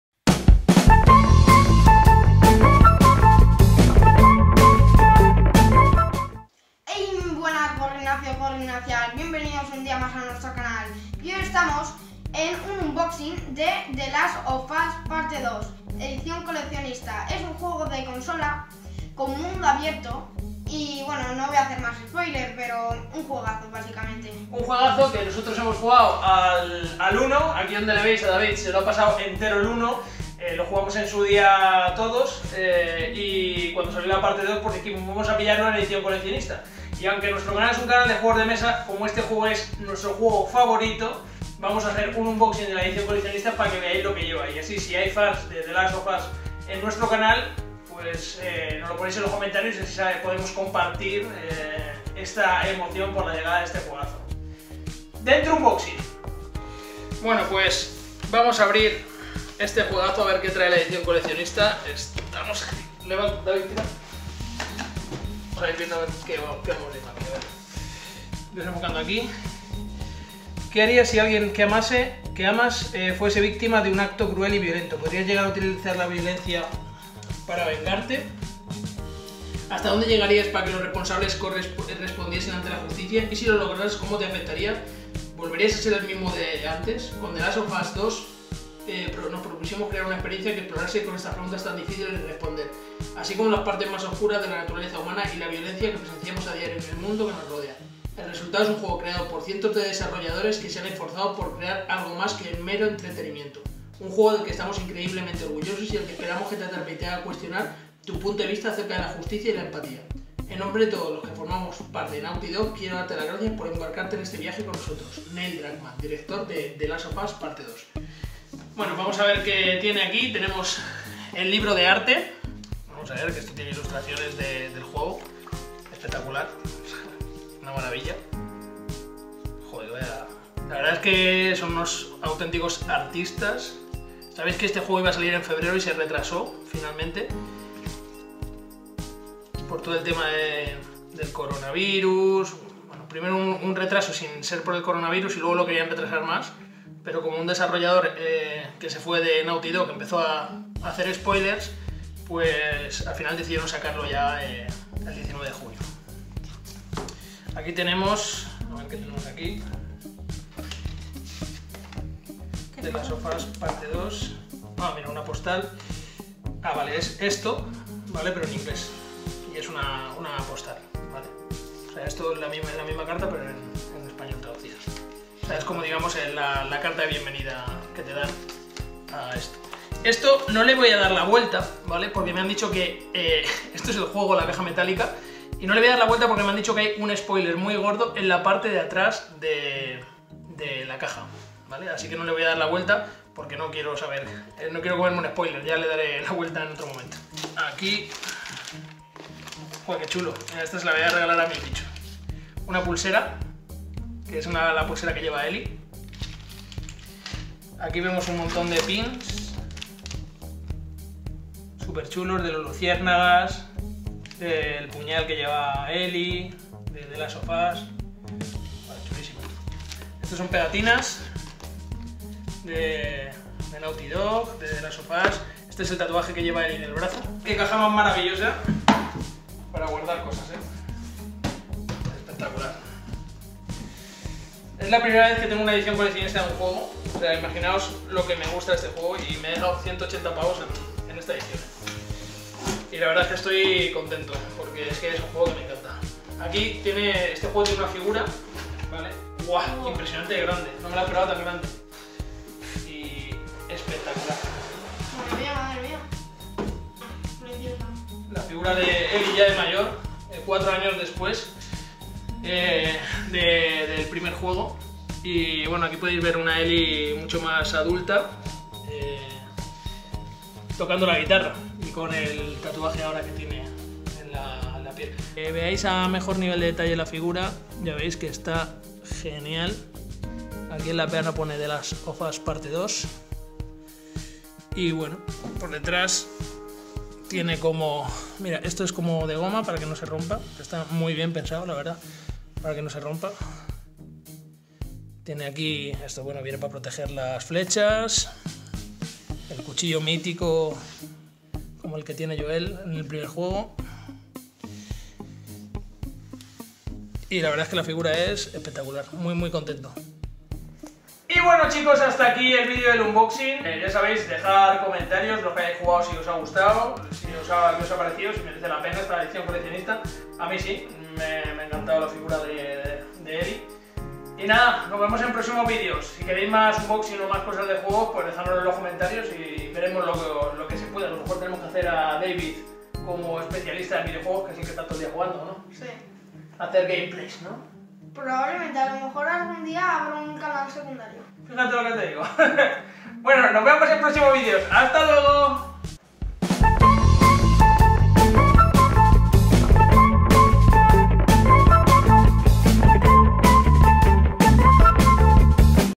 The Last of Us (Parte 2): Unboxing | Edición Coleccionista
Aunque sabéis que este canal es de juegos de mesa, y siendo este nuestro juego preferido, no queríamos perder la ocasión de enseñaros la copia que hemos comprado del Juego de PS4: The last of us parte 2 edición coleccionista. ¿Habéis jugado al 1?¿Os está gustando el juego?¿Sois tan fans como nosotros? Déjanos un comentario :)